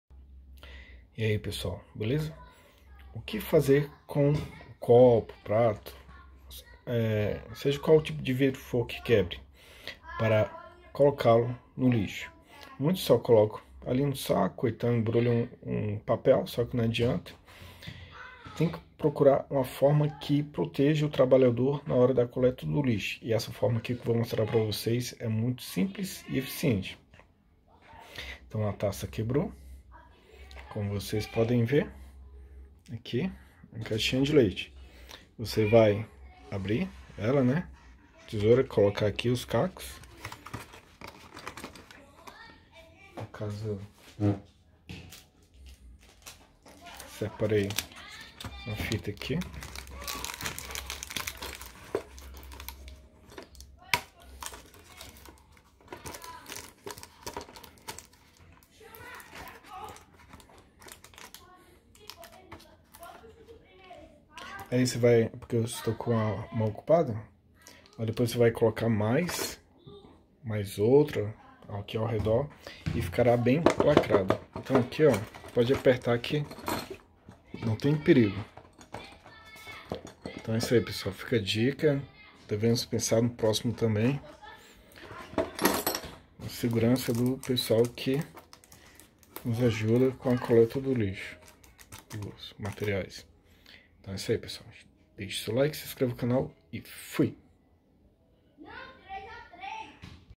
COMO PROTEGER OS COLETORES DE LIXO DE CACOS DE VIDRO
Dica simples e eficiente.